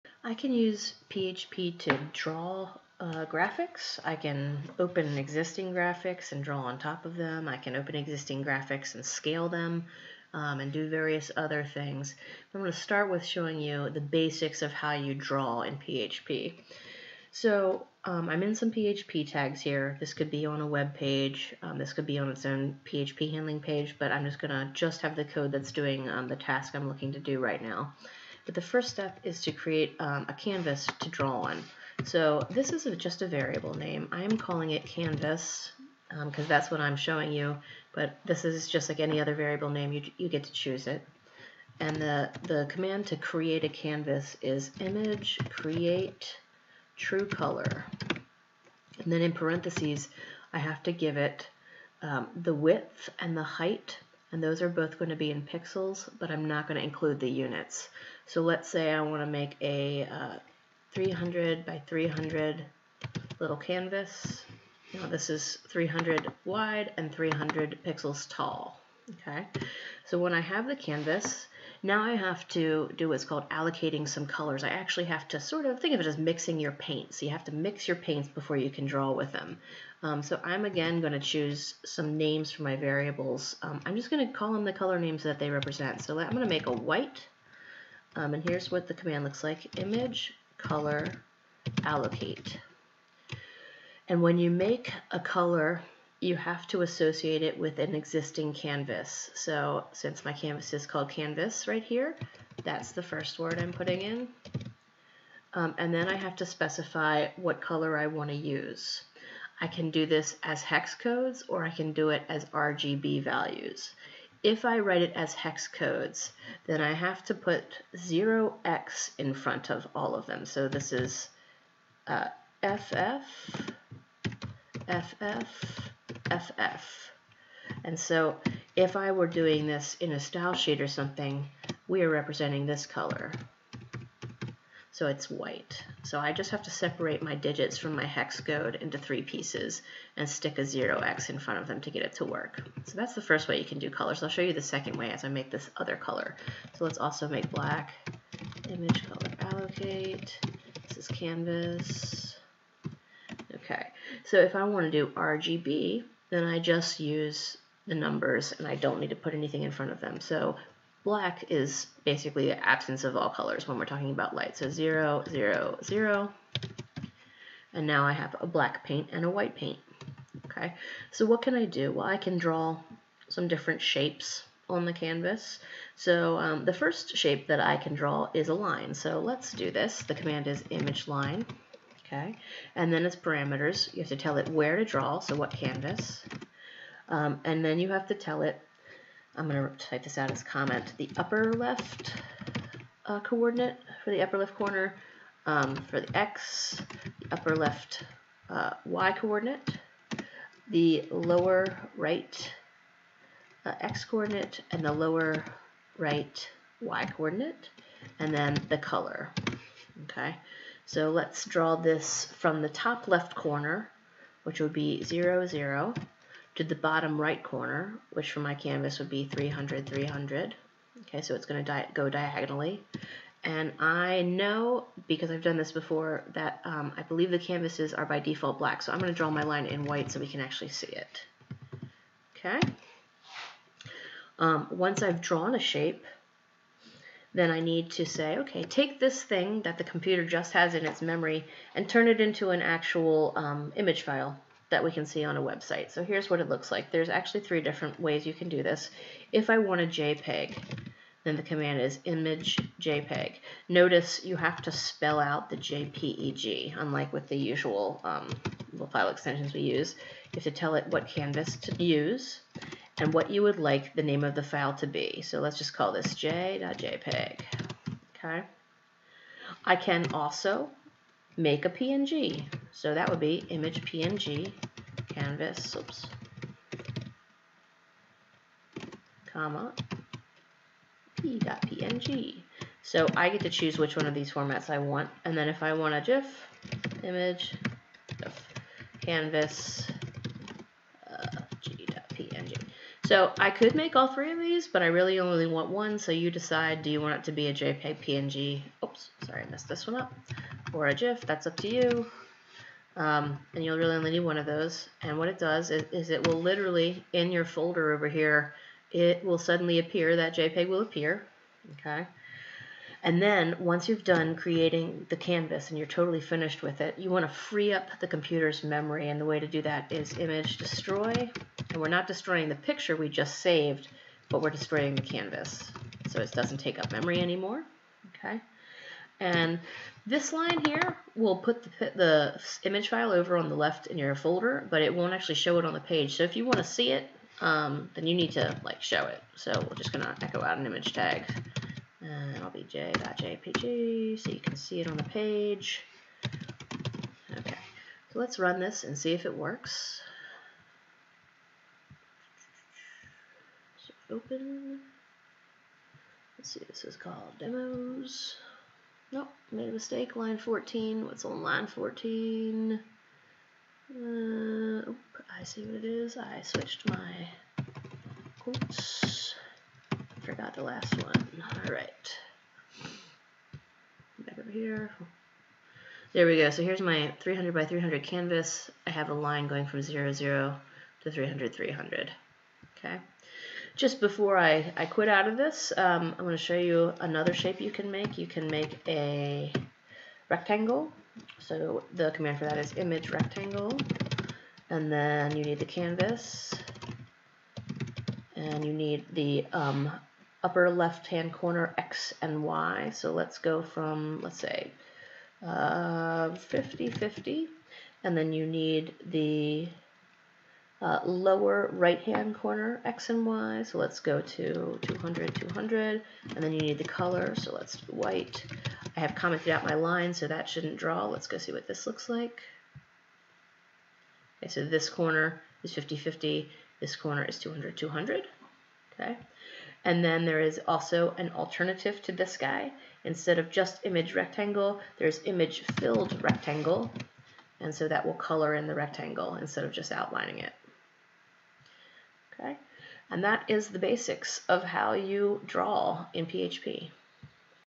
PHP Basics of Drawing
How to make a canvas, allocate colors, draw lines, draw rectangles, write to file, destroy, show on page in PHP.